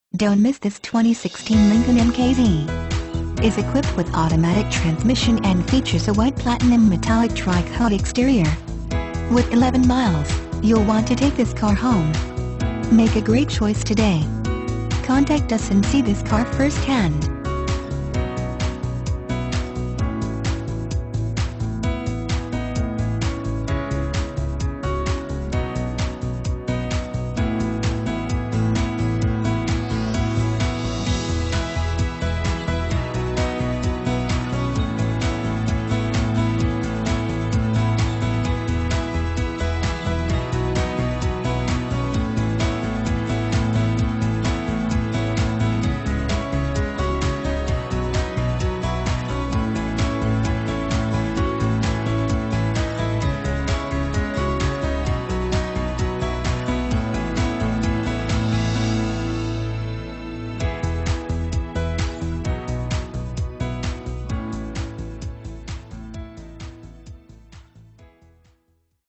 2016 Lincoln MKZ 160113
2016 LINCOLN MKZ Merritt Island, FL
Stock# 160113

Island Lincoln
1850 East Merritt Island Cswy

You'll love this 2016 Lincoln MKZ. This is a car you'll want to take home. With 11 miles, it features Automatic transmission and an exterior color of White Platinum Metallic Tri-Coat. Call us and be the first to open the car door today!